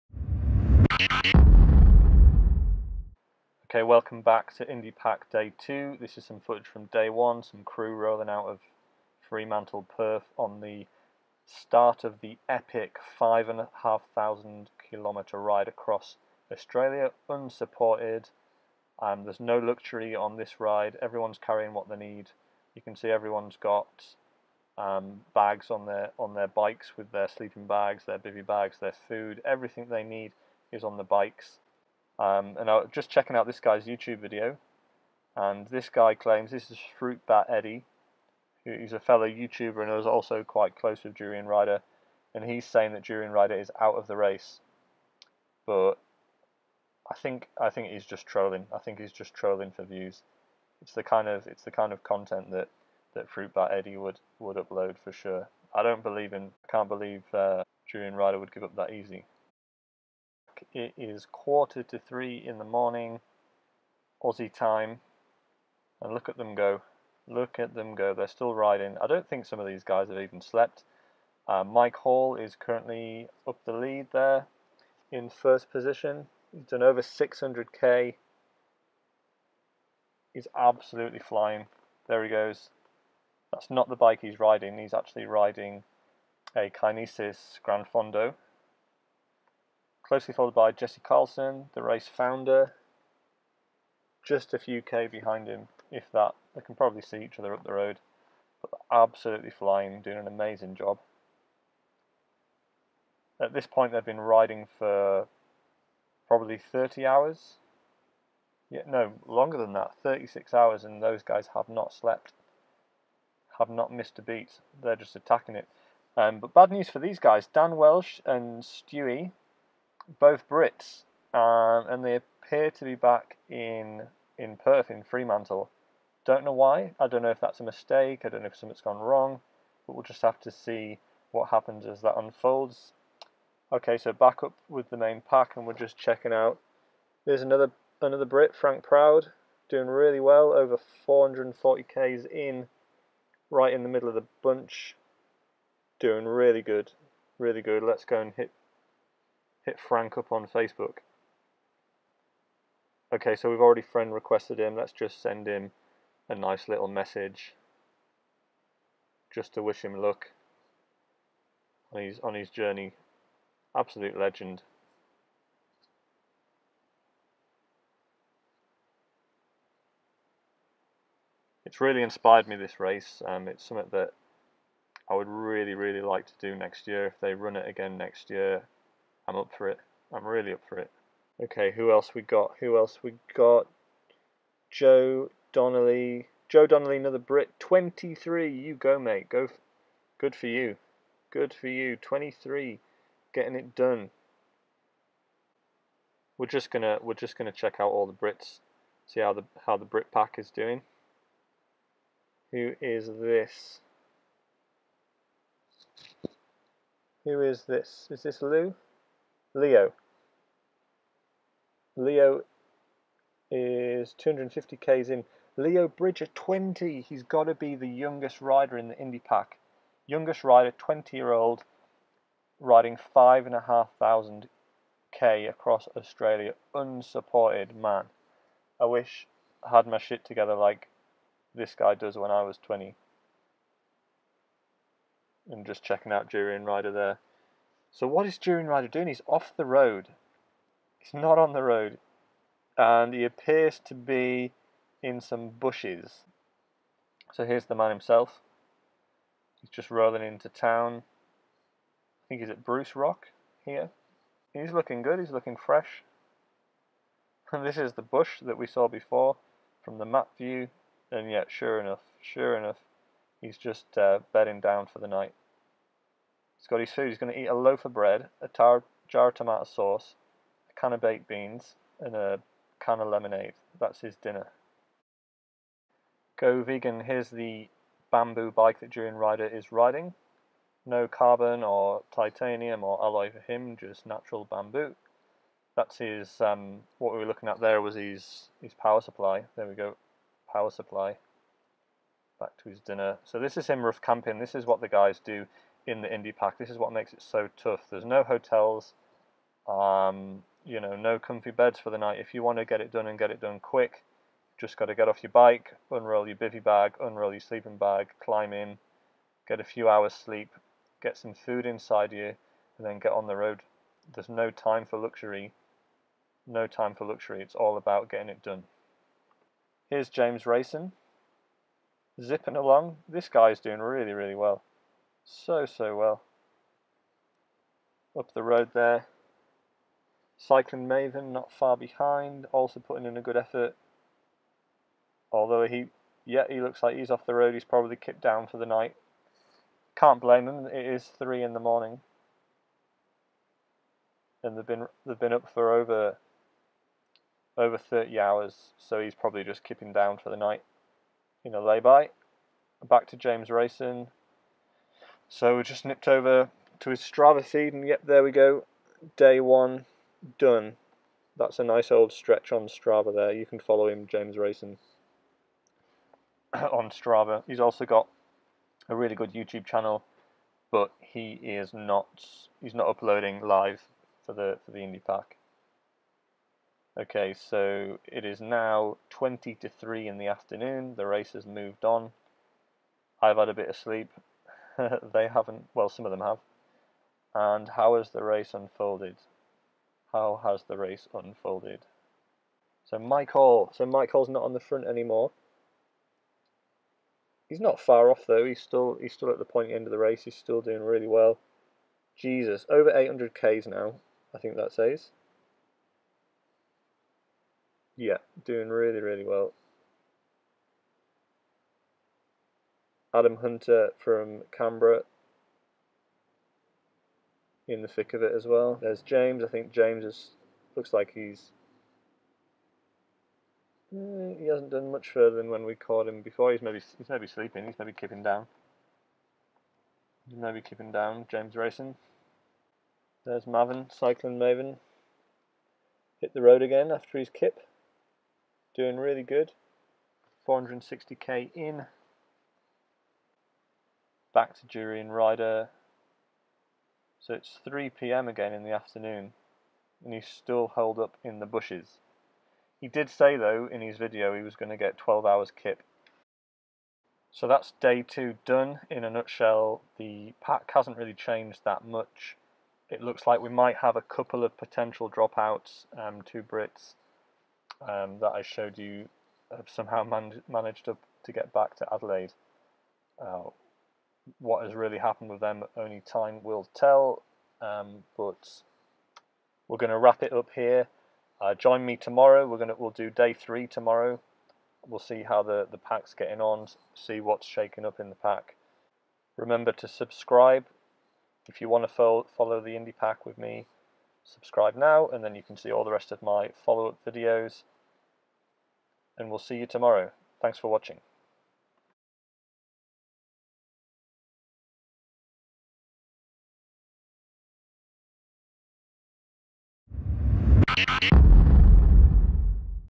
Indian Pacific Wheel Race day 2 Durianrider, Stu McCormick, Dan Welch out of race?
Indian Pacific Wheel Race day 2, Durianrider, Stu McCormick, Dan Welch out? Indipac

Day 2 of Indipac, Durianrider and Jesse Carlsson are out of the race. Follow the other competitors as they compete in the toughest bike race on earth, the Indian Pacific Wheel Race from Perth in Western Australia ending in Sydney.

Day 2 of Indian Pacific Wheel Race IPWR and we take a look at Australia vs Britain!!

Longer tougher and more rugged than the trans american bike race and featuring some former trans am competitors and winners!

Who are you cheering for in the toughest endurance bike race on earth?

A revue and commentary of Indipac an ultra endurance bicycle race across Australia from Fremantle, Perth to Sydney.

Competitiors include Durianrider, Mark Ferguson aka Cycling Maven, James Raisson aka ride adelaide, Jesse Carlsson, Mike Hall, Donncha Cuttriss, Kristoff Allegaert, Sarah Hammond

Take a look at curve cycles, there belgie spirit and belgie classic vs Kinesis Gran Fondo TI V3

related channels